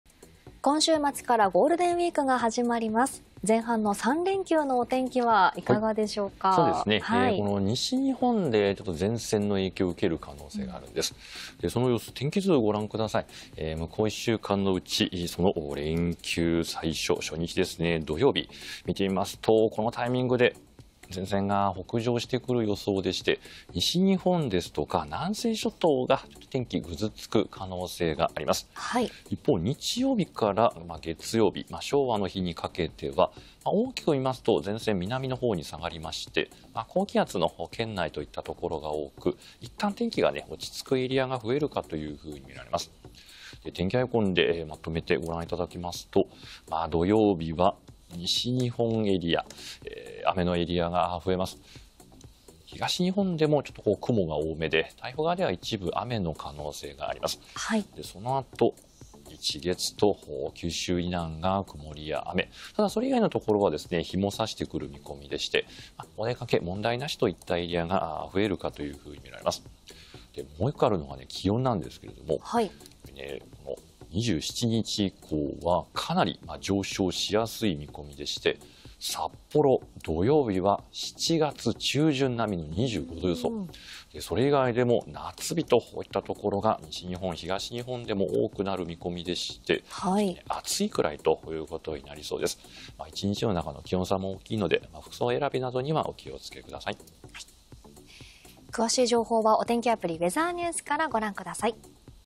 【GWスタート】前線の影響で西日本の太平洋側で雨
早い方はGWが始まりとなる三連休初日27日(土)は、本州の南に停滞する前線が北上傾向で、東海以西の太平洋側を中心に雨の降るところが多くなりそうです。